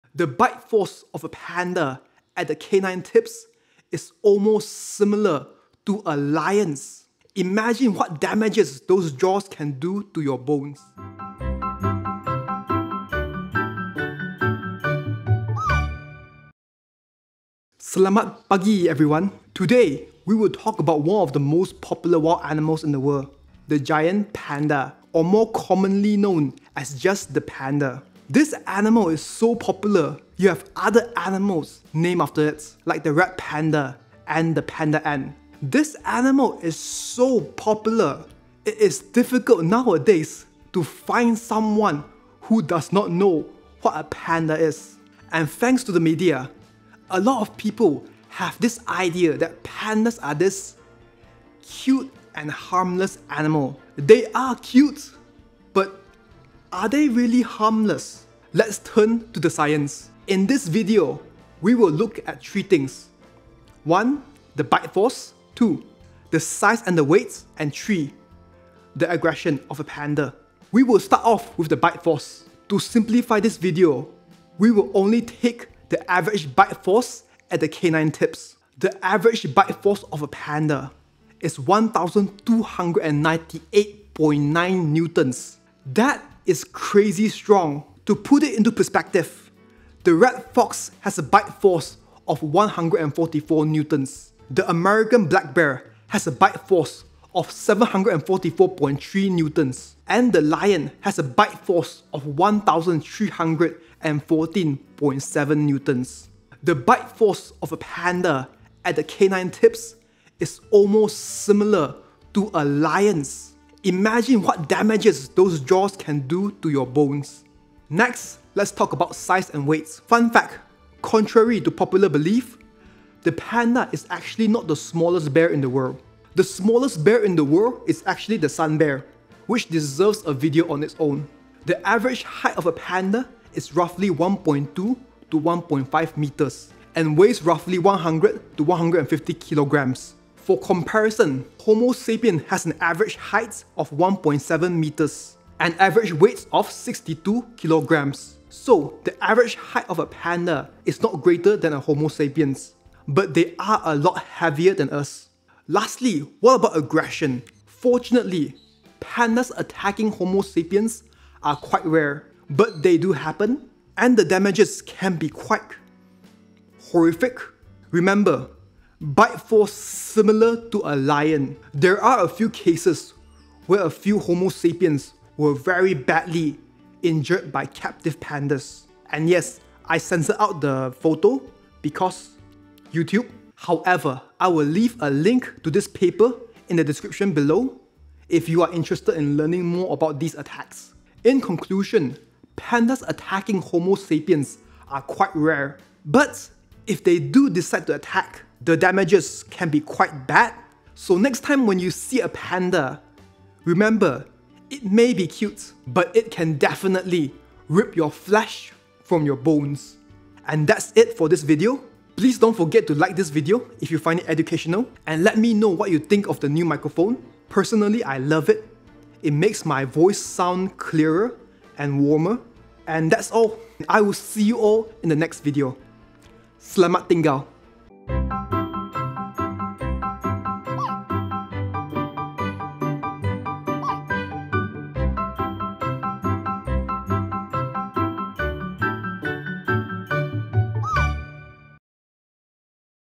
Are Pandas Dangerous? Yes, yes they are.
Giant Pandas  (Ailuropoda melanoleuca) are one of the most population wild animals in the world. With software named after the Giant Pandas like "Panda Helper ord"  and the rise of panda cam like the "Smithsonian Panda Cam" and the "Giant Panda Cam", I will not be surprised if the Giant Pandas' popularity grows exponentially in the next few years!

Cuteness aside, we have to ask ourselves, are Panda dangerous? Or just how dangerous are Pandas? Have Pandas killed or injured anyone before? Short answer is yes, they are dangerous. The bite force of a Giant Panda is similar to a Lion's bite force! Long answer is it depends. How threatened does the panda feel? How much did you annoy the Panda? The details will be discussed in this video.

REFERENCES

Charles J. Sharp (2014). African Lion, Phinda Private Game Reserve, KwaZulu Natal, South Africa. Commons.wikimedia.org/wiki/File:Lion_(Panthera_leo)_male_6y.jpg

Chris Lukhaup (2015). Panda Ant. Commons.wikimedia.org/wiki/File:Hormiga_panda.png.

Christiansen, P. and Wroe, S. (2007). Bite forces and evolutionary adaptations to feeding ecology in carnivores. doi: 10.1890/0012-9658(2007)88[347:BFAEAT]2.0.CO;2.

Greg Hume (2010). American black bear - Ursus americanus - detail of head. Taken at Cincinnati Zoo. Commons.wikimedia.org/wiki/File:Ursus_americanusDetail.jpg.

Mattis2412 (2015). Red Panda. Commons.wikimedia.org/wiki/File:Red_Panda_04.jpg.

Nat Geo (n.d.). Giant Panda. Nationalgeographic.com/animals/mammals/facts/giant-panda.

Nakonechnyj, A. (2010). Bamboo Forest. Retrieved from Flickr.com/photos/alexnako/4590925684.

Peter G Trimming (2011). Yawning Red Fox. Commons.wikimedia.org/wiki/File:Yawning_red_fox.jpg.

Peter, T. (2016). Panda-hug. Retrieved from Commons.wikimedia.org/wiki/File:6990634-panda-hug.jpg.

Pier, J. (2009). When Pandas Attack. Retrieved from Flickr.com/photos/jasonpier/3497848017.

Regis, E. (2008). IMG_0985. Retrieved from Flickr.com/photos/30271386@N04/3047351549.

Roser et al. (2013). Human height. Ourworldindata.org/human-height.

Rushenb (2020). Sun bear in wild, Kaeng Krachan National Park, Thailand. Commons.wikimedia.org/wiki/File:Sun-bear.jpg.

Sheilalau (2006). Panda Cub from Wolong, Sichuan, China. Commons.wikimedia.org/wiki/File:Panda_Cub_from_Wolong,_Sichuan,_China.JPG.

Roger Teo. (2021). Jason The Zoologist, giant panda, rhinoceros hornbill, campfire and fig cartoons. Mochifox.com.

Walpole, et al. (2012). The weight of nations: an estimation of adult human biomass. doi: 10.1186/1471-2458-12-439.

WWF (n.d.). Giant Panda. Worldwildlife.org/species/giant-panda.

Zhang et al. (2014). Three cases giant panda attack on human at Beijing Zoo. Ncbi.nlm.nih.gov/pmc/articles/PMC4276236/.